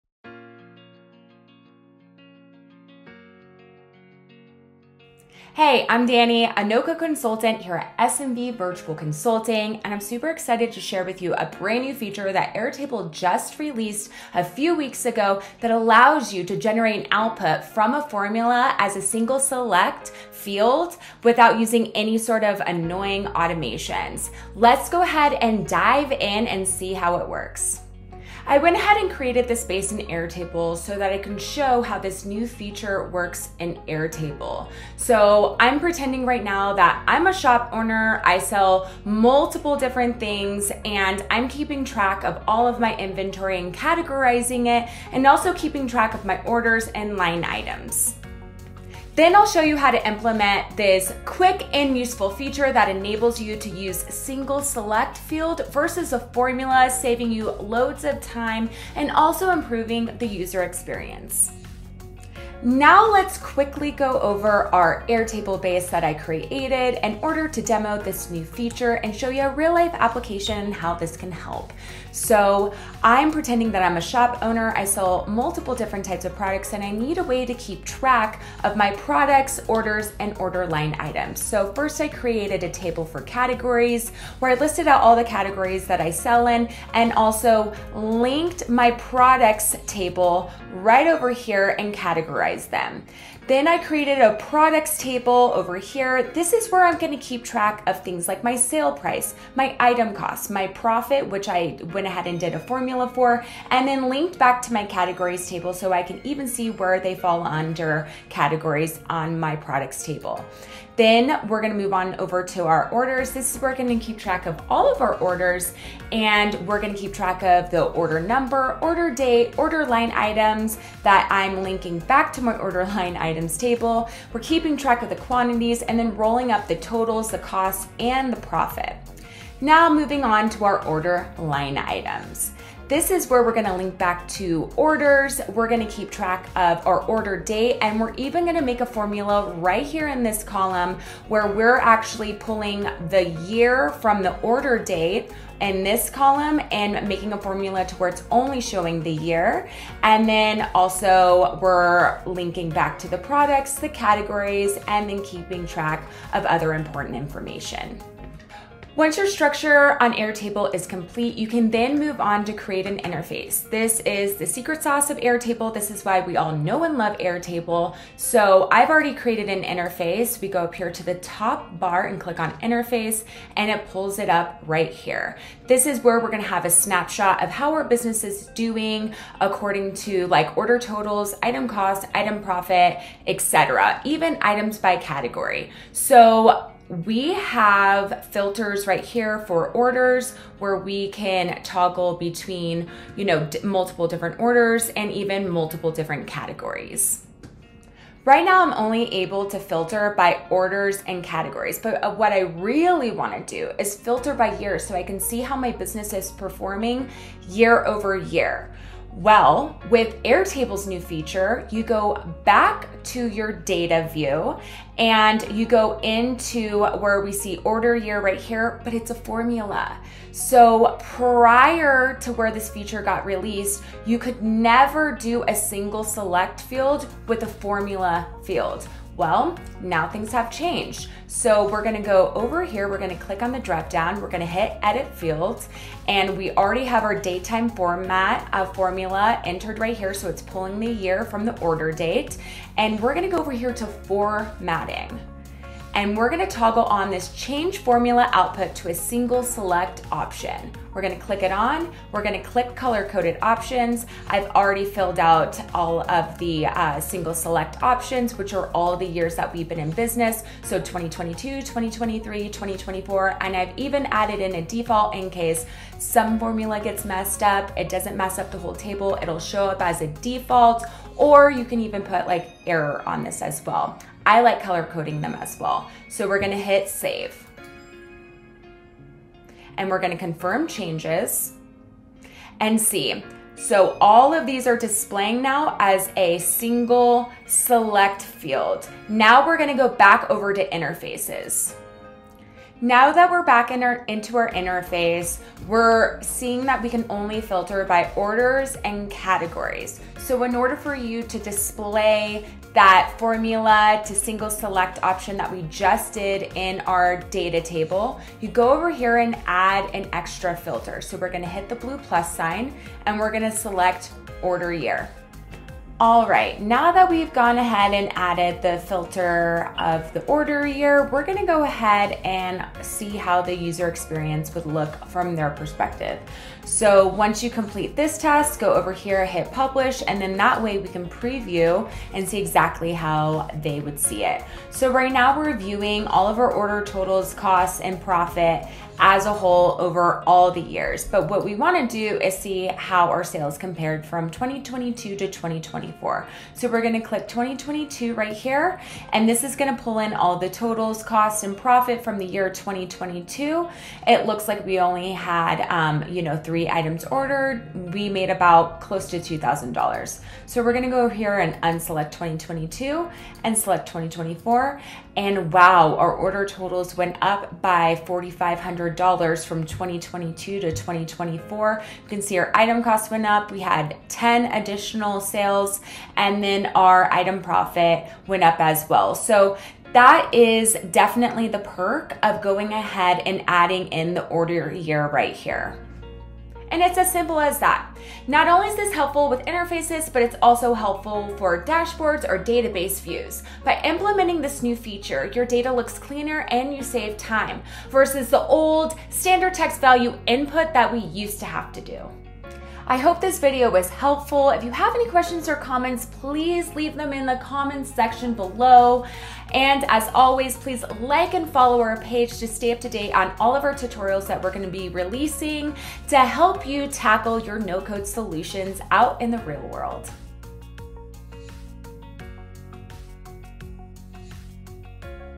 Single Select Output Revolutionizes Formula Fields!
Welcome to our tutorial on how to use the newest upgrade on Airtable: Formula to Single Select Field.

In this video, we'll dive into the exciting new feature announced by Airtable, where you can now convert formula outputs directly into single select fields. This enhancement is particularly beneficial for creating more dynamic and interactive interfaces, but it can also be incredibly useful in your dashboards and various database views.

By the end of this video, you'll have a solid understanding of how to implement the new Formula to Single Select feature, enabling you to streamline your data management and enhance your Airtable experience. Whether you're a beginner or an experienced user, this tutorial will guide you through the setup and show you practical examples of how to leverage this update to its fullest potential.

Timestamps:

00:00 Intro
01:00 Its all about that Base
02:47 Your interface
03:51 How to turn your formula into a single select field.
04:52 Filtering by your formula single select field.
05:34 Preview your final product
06:26 Outro

 / smv-virtual-consulting

If you have any questions, leave them in the comments below. Happy form building!